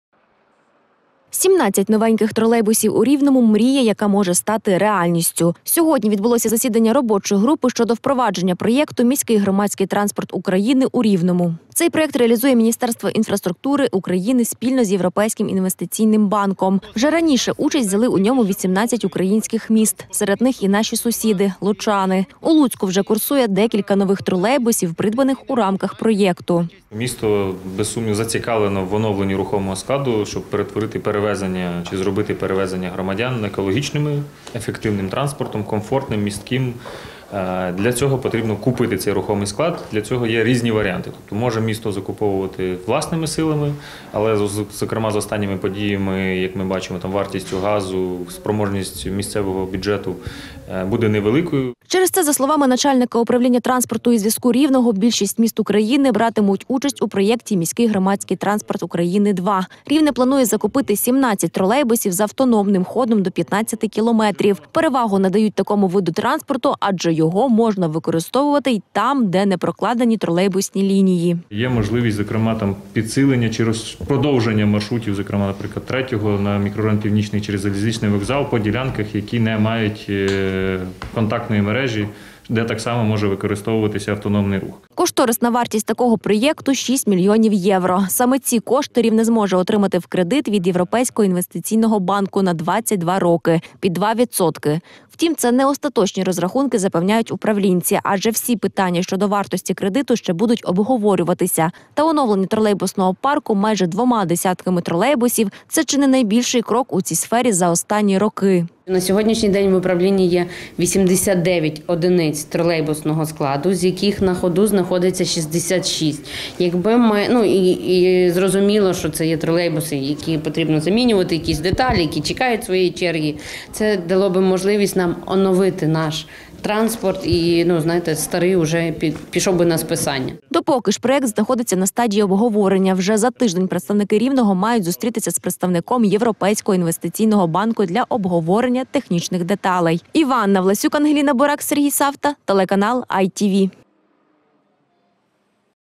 Нові тролейбуси у Рівному: мрія може стати реальністю?
Тролейбусний парк Рівного незабаром може оновитися. Адже наше місто хоче брати участь у проєкті, який ініціює Міністерство інфраструктури України. Наразі міські управлінці на етапі обговорення. Сьогодні вже відбулася перша зустріч. Деталі знають наші журналісти.